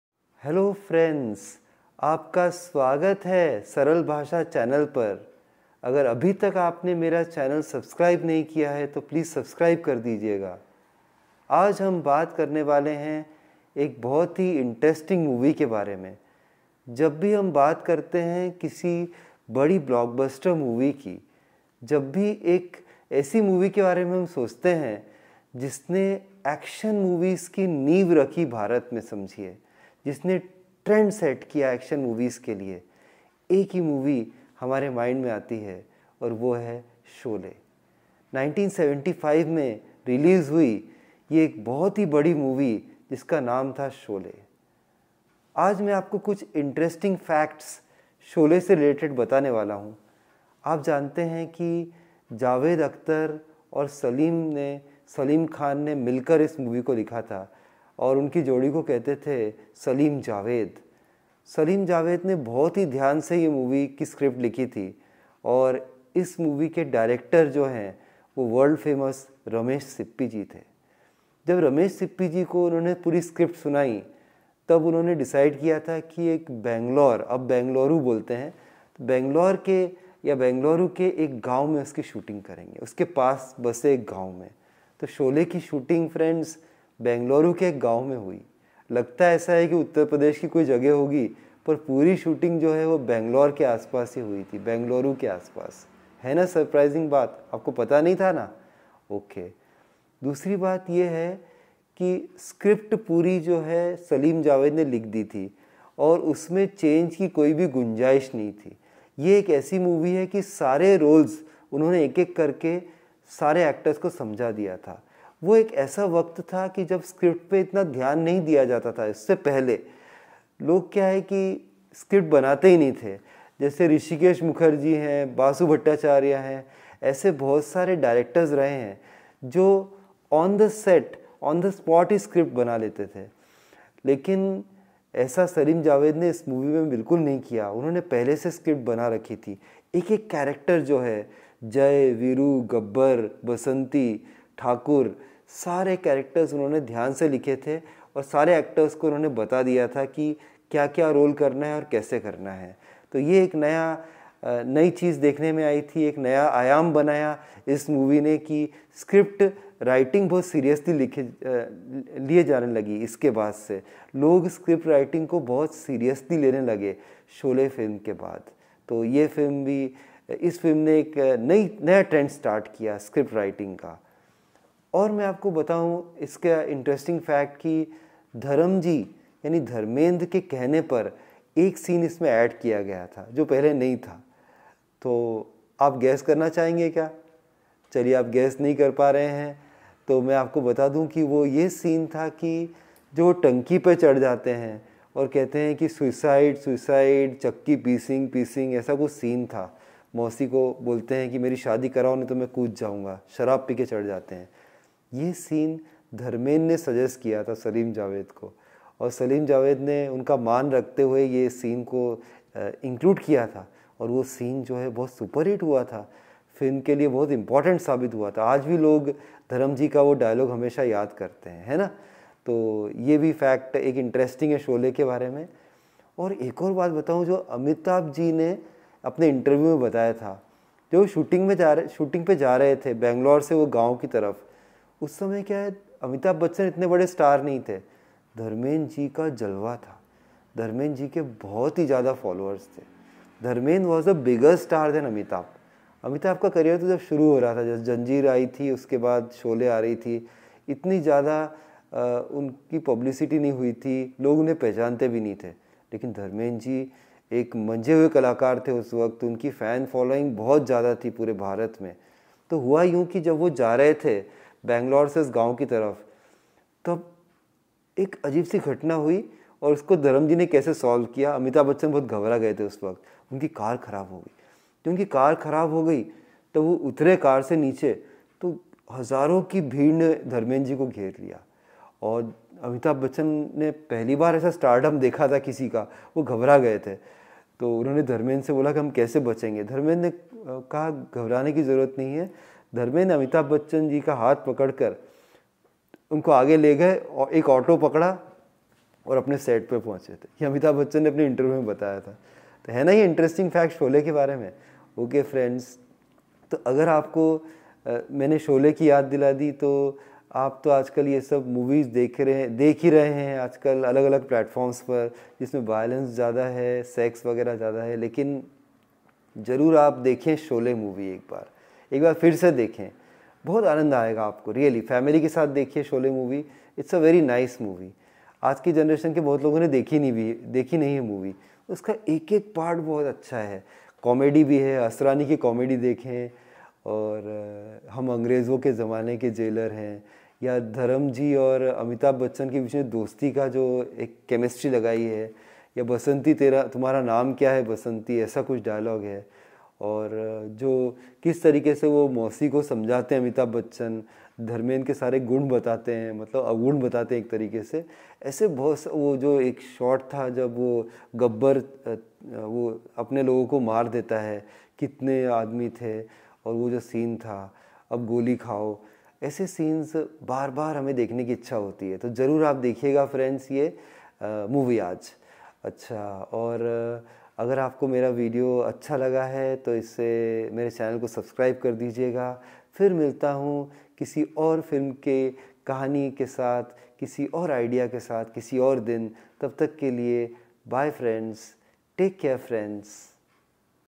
शोले मूवी के अनजाने राज
शोले मूवी के अनजाने राज। interesting facts about Sholay movie.